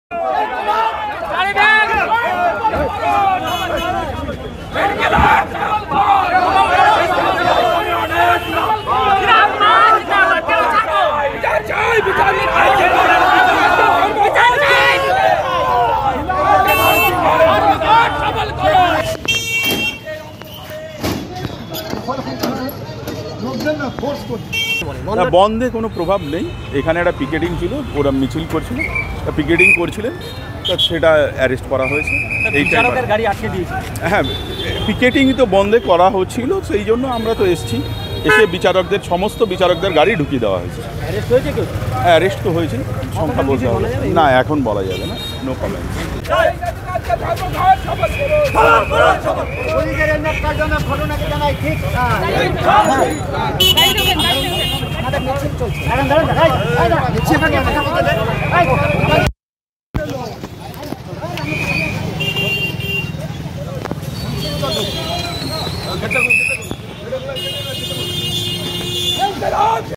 SUCI-এর ডাকে ১২ঘণ্টার বাংলা বনধ, বিচারকের গাড়ি আটকানোকে কেন্দ্র করে জলপাইগুড়িতে ব্যাপক উত্তেজনা
পুলিশের সঙ্গে ধস্তাধস্তি এসইউসিআই কর্মী-সমর্থকদের